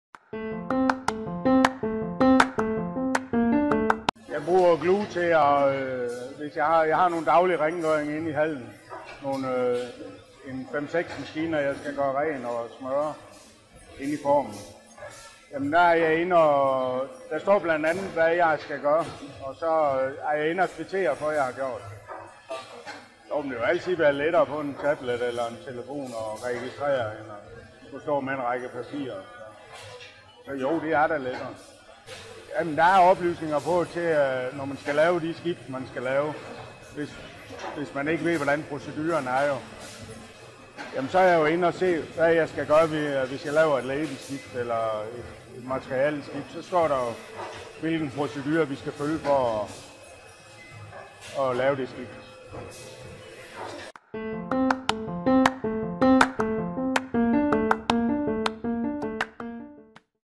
How does a Machine Operator use Guuu?
A learning case from Superfos, a Danish manufacturer of sustainable plastic solutions. Listen to Ole Brøker Thomadsen, a Machine Operator, explain how he uses Gluu to complete daily tasks on the production floor.

All screen recordings are only examples, they are not live processes of Superfos.